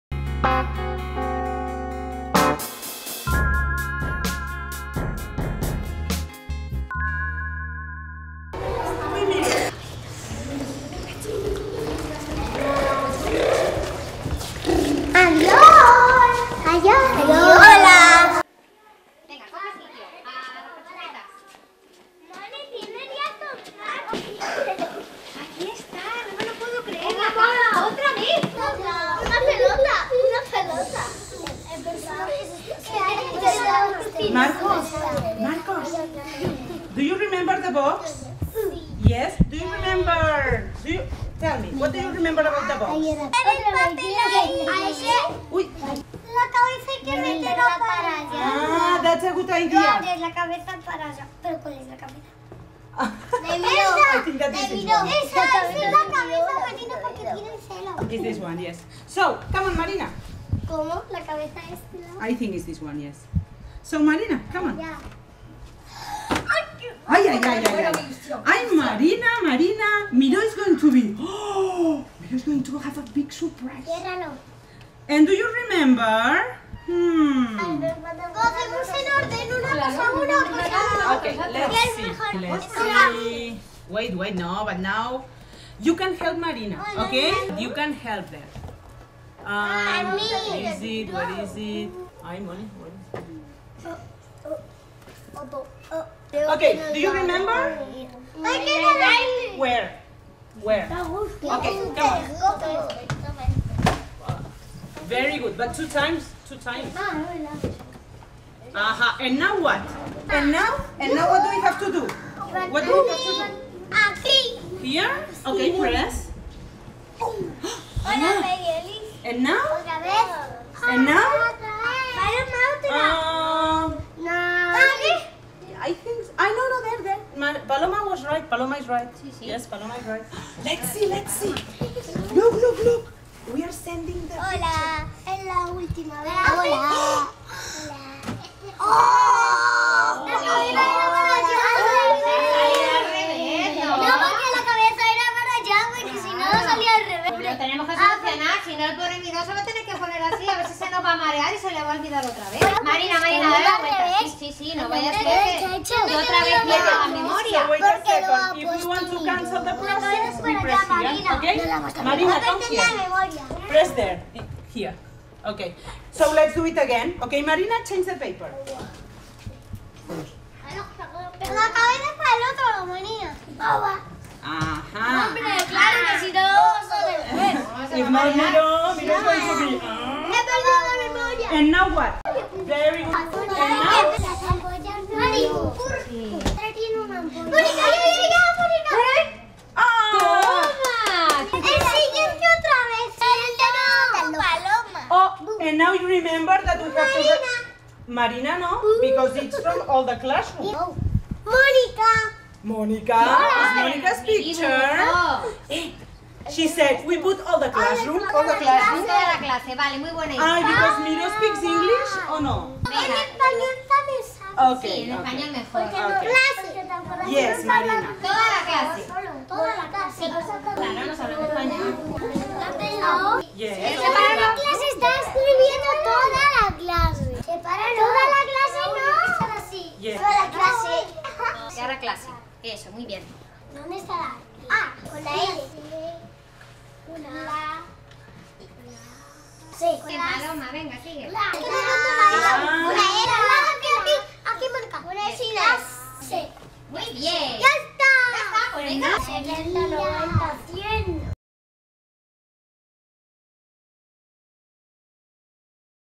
Usando el escáner
El uso de las tecnologías está presente a lo largo de todo el proyecto, pero sobre todo a la hora de relacionarse con el personaje.